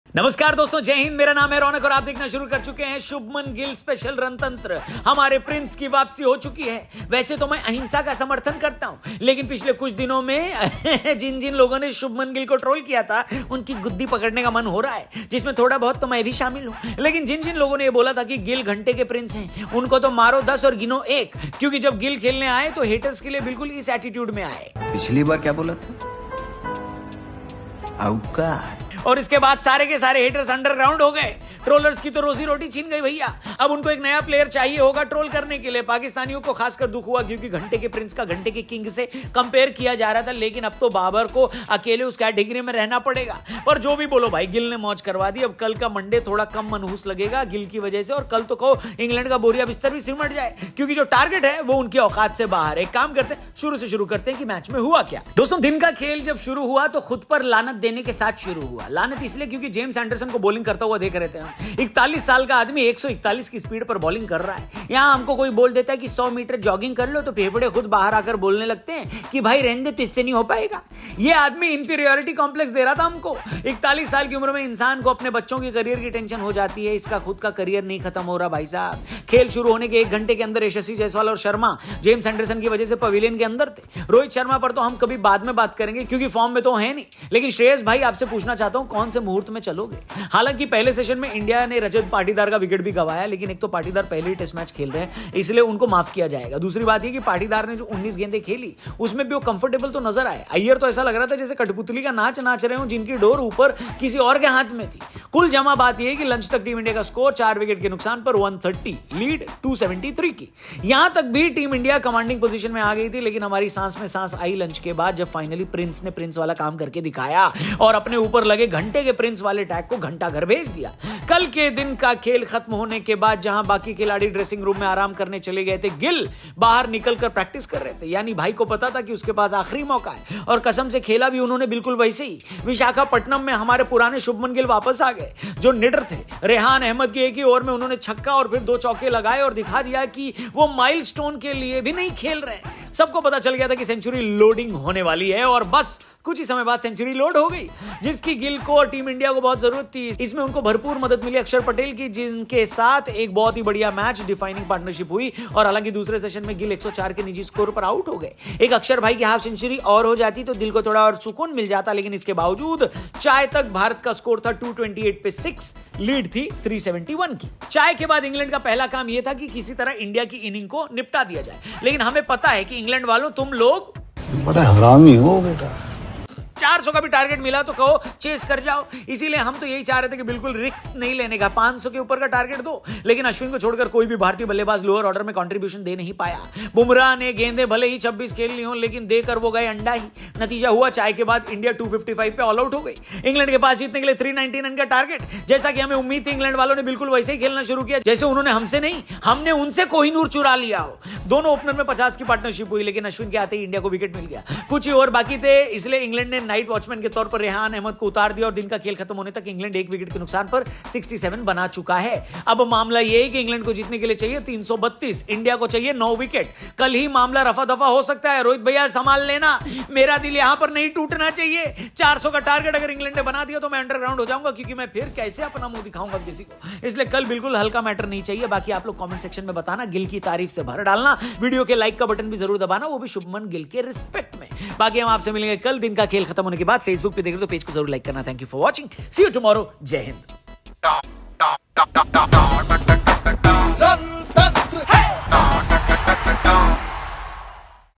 गिल की सेंचुरी से मजबूत पोजीशन में भारत. लेकिन क्या मैच जीत पाएगा? | IND vs ENG | Rj Raunak
Photo Credit: AFP